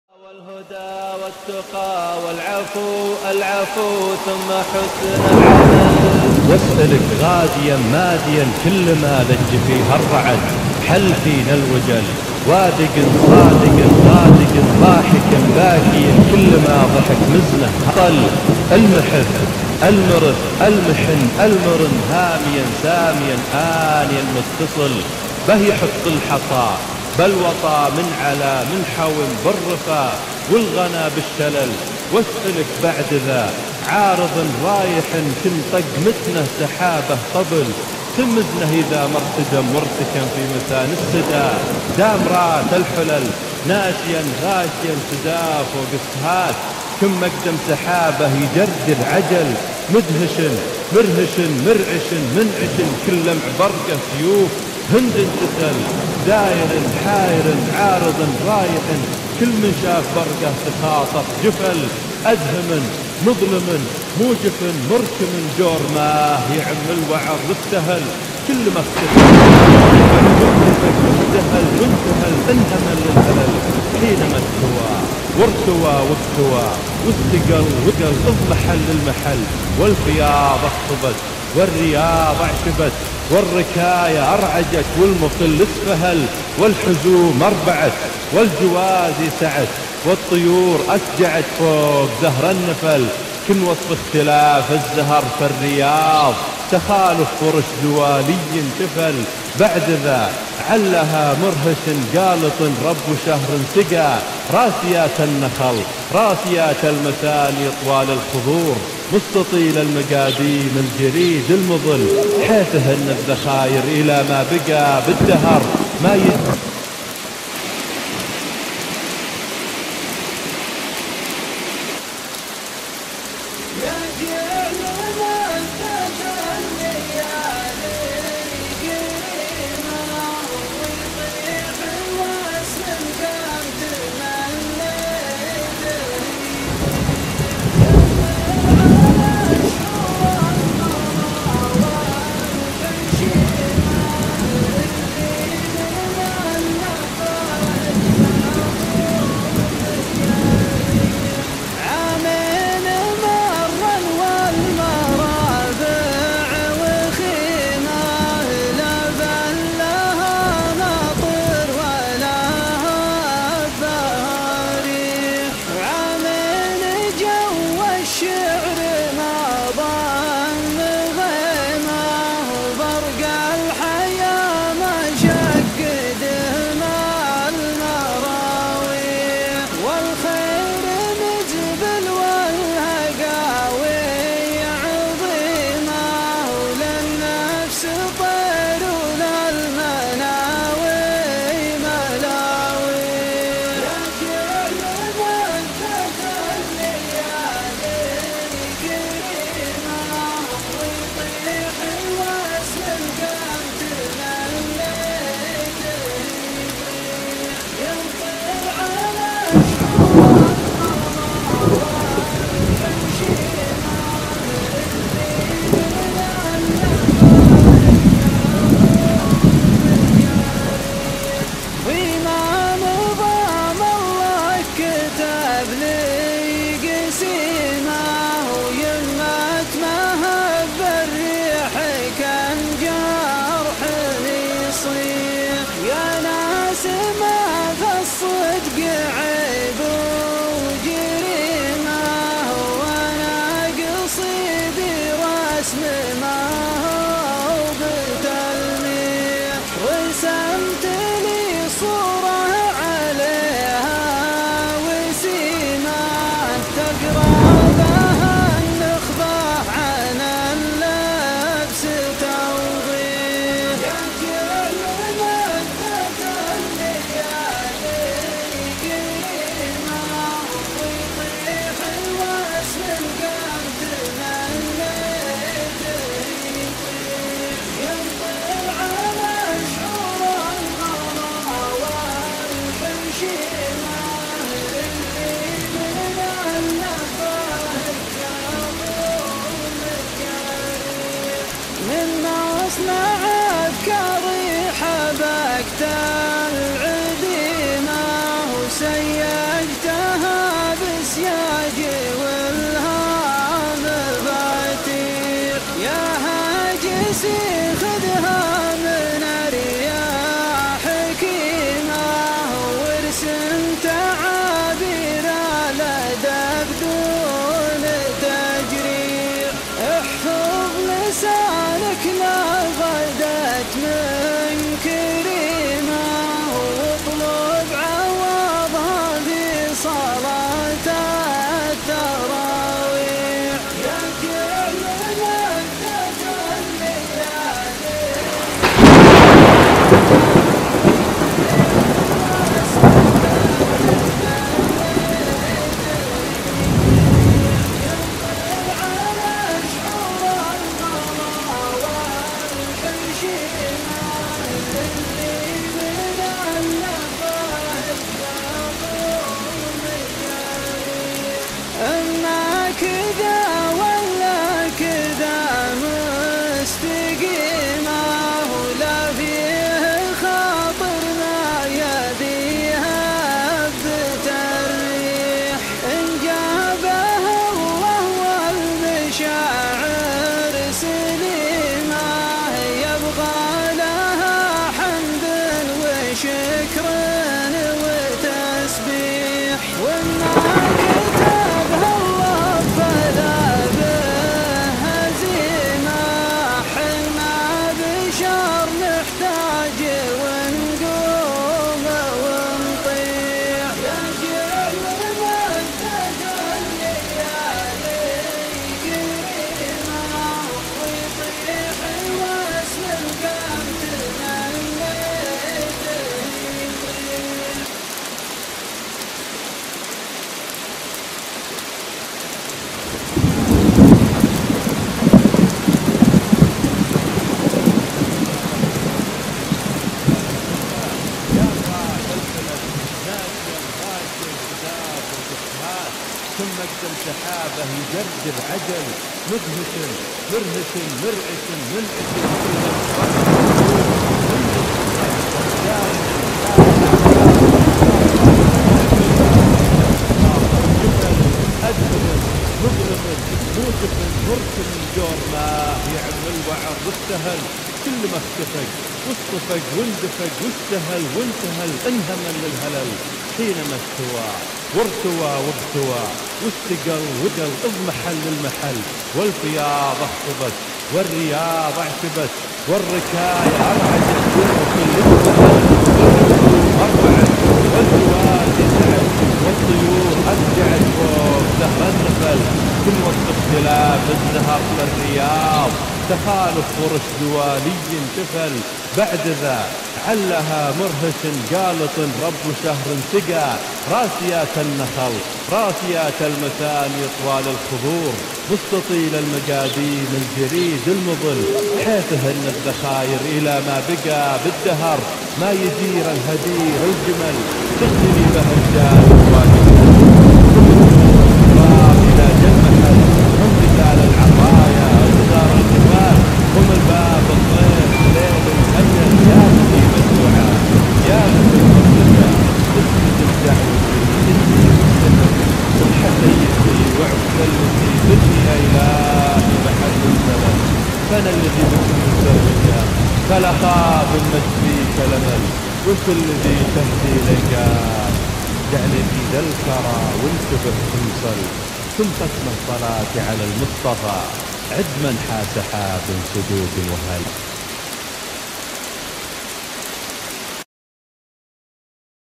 تصوير جوي لسيل روضة سدير الخميس 13-6-1439هـ
وادي سدير وادي الفقي وادي الجوفاء بروضة سدير
عدسة خالدب بن عبدالله الفارس alfaris15